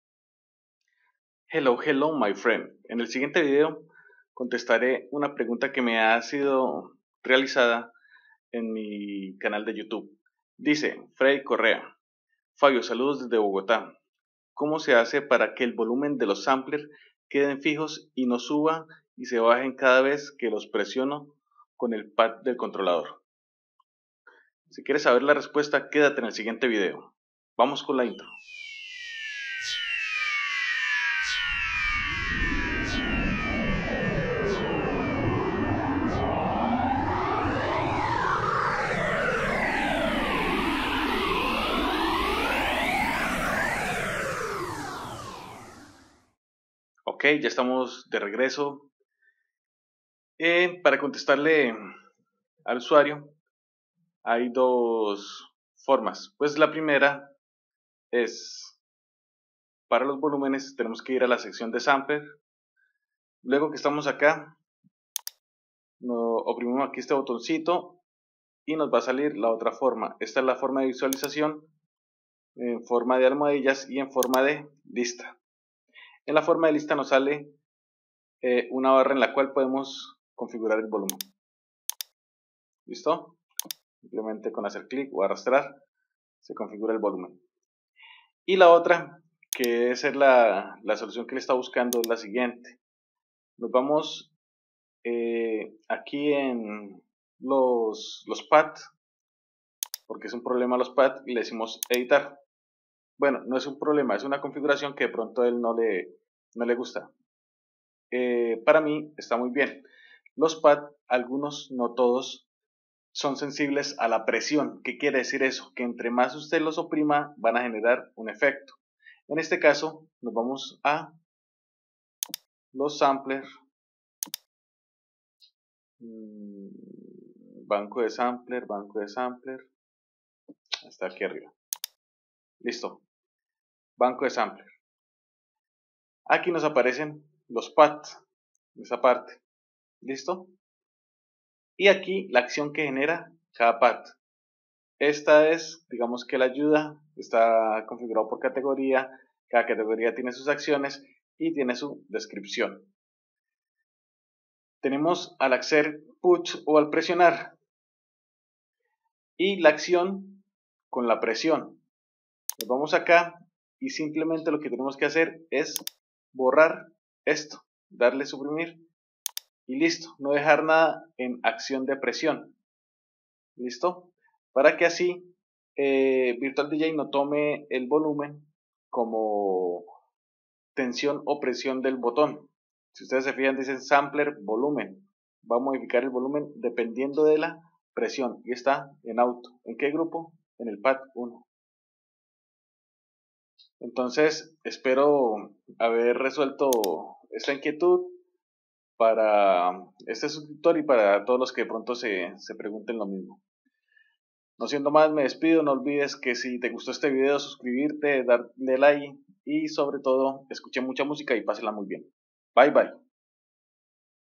modificar la presión de los sampler en los pad de virtual dj 8
configuración avanzada de los pad´s en la controladora